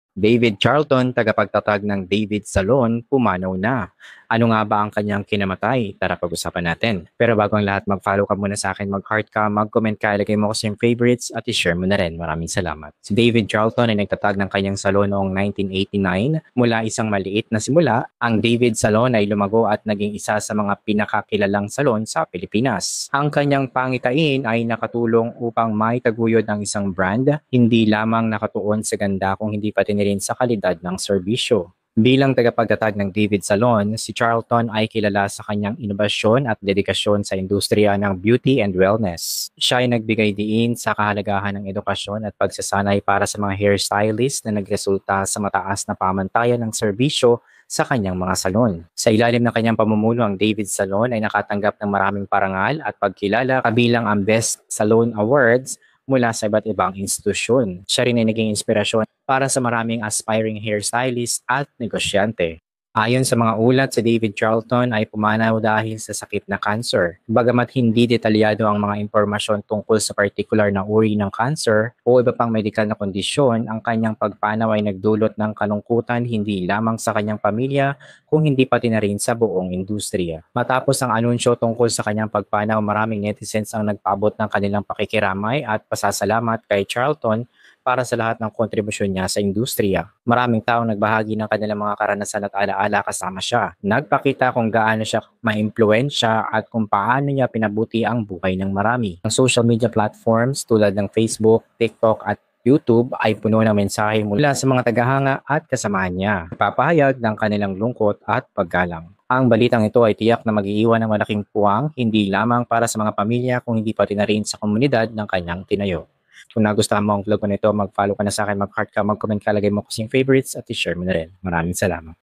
DAVID CHARLTON PUMANAW NA | DAVID’S SALON FOUNDER
Today, we honor David Charlton, the innovative founder of David’s Salon. His enduring legacy of creativity, commitment, and exceptional service will remain a source of inspiration for us all. He will be profoundly missed.
Photo credits: L'Oreal Professional